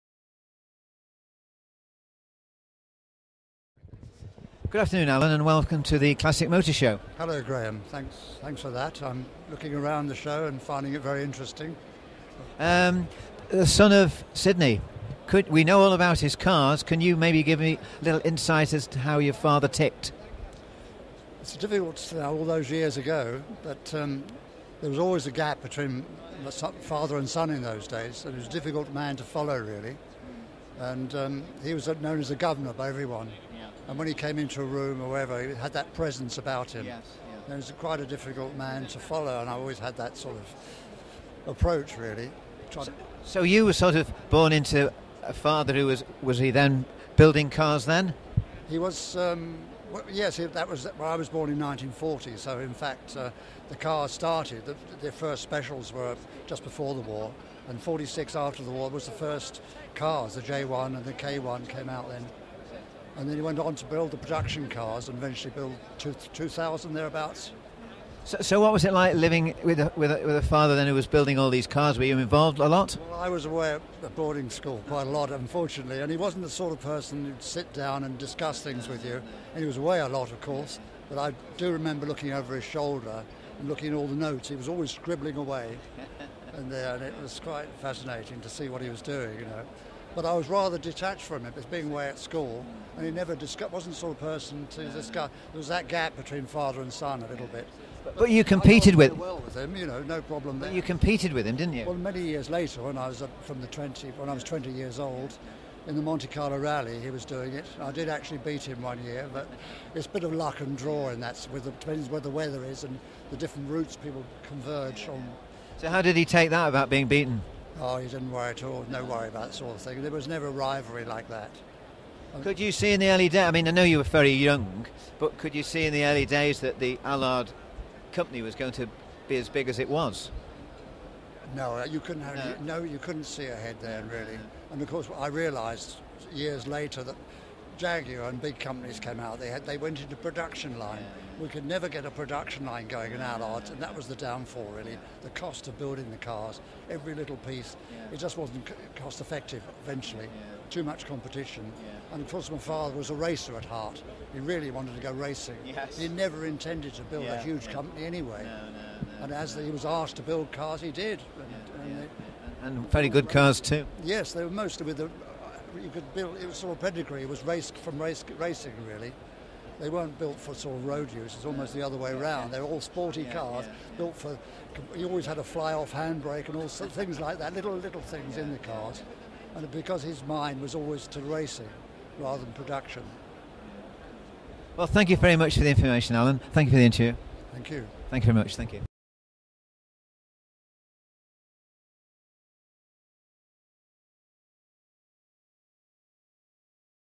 Allan Allard Interview | Allard Owners Club Stand | Classic Motor Show 2016
This video has Graham Clark of VFS Videos interviewing Sydney Allard's son Allan on the Allard Owners Club Stand at the NEC Classic Motor Show Sunday 13th November 2016.

Allan talks about living with his father, his father's cars and competing against him in the Monte Carlo Rally.